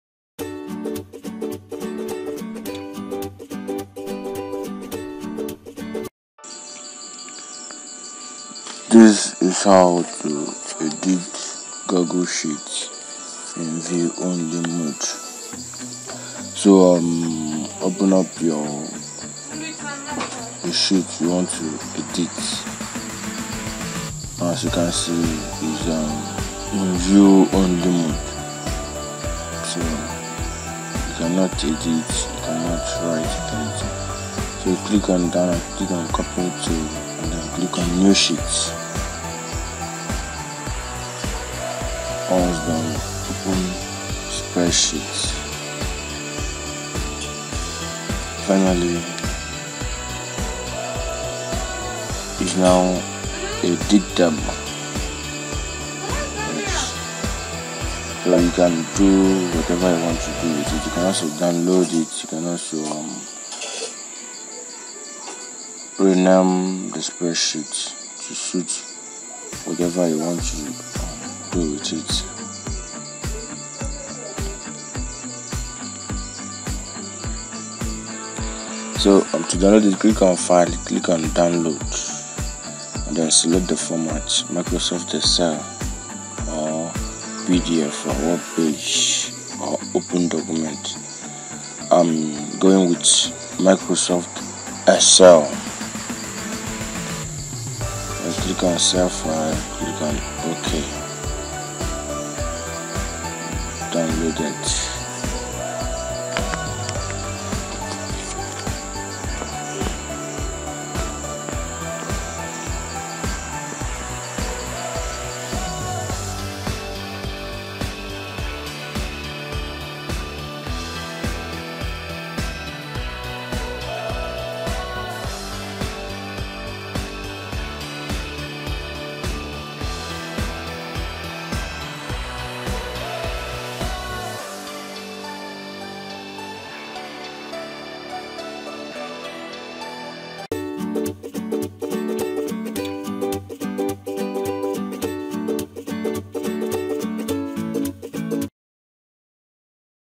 How to Edit Google Spreadsheet in View Only Mode
Unlock and edit your Google spreadsheet in view only mode you will also learn how to download the spreadsheet in different format.